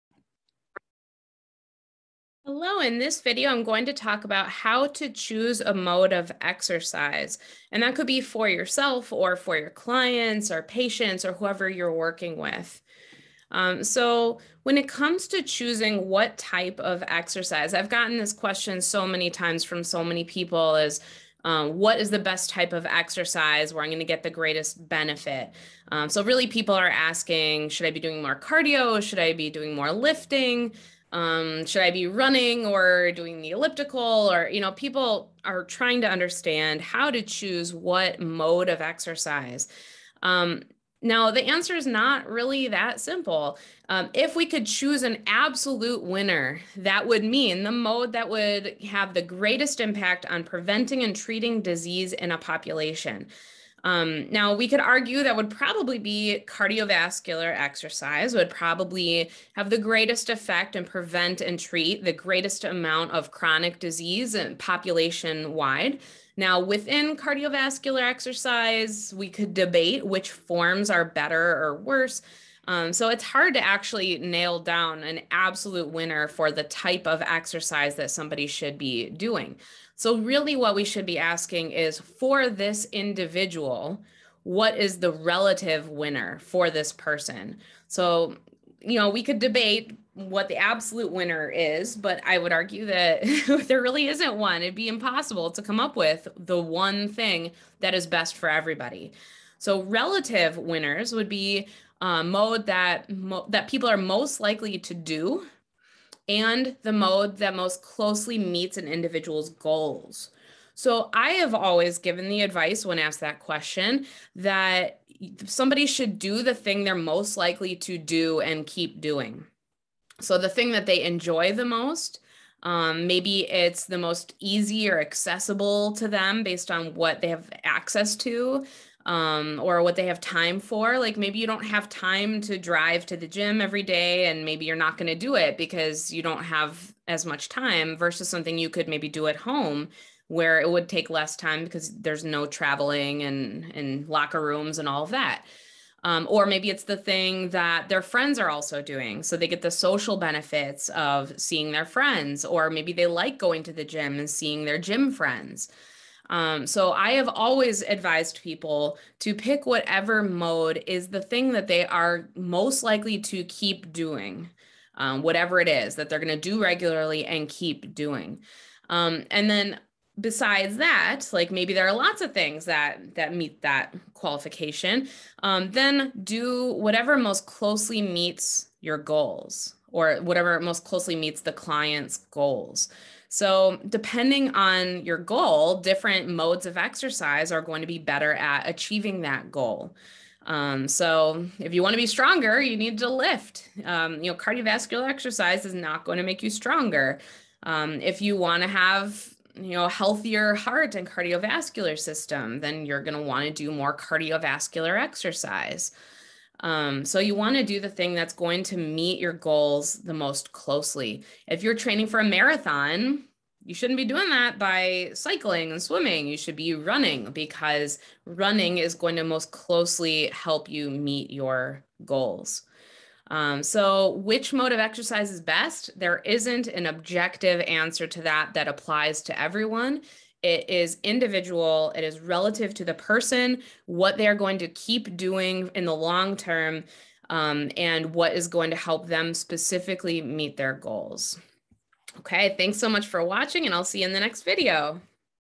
Choosing a Mode of Exercise
Choosing a Mode of Exercise: Exercise Prescription, Type, Mode, Absolute winner, Relative winner, Goals

References

Ehrman, J. K., Gordon, P. M., Visich, P. S., & Keteyian, S. J. (2023). Clinical exercise physiology, (5th ed.). Human Kinetics.

Magyari, P., Lite, R., Kilpatrick, M. W., & Schoffstall, J. E. (2018). ACSM’s resources for the exercise physiologist, (2nd ed.). Wolters Kluwer.

Riebe, D., Ehrman, J. K., Liguori, G., & Magal, M. (2018). ACSM’s Guidelines for exercise testing and prescription. Wolters Kluwer.

Woolf-May, K. (2006). Exercise prescription: Physiological foundations. Elsevier.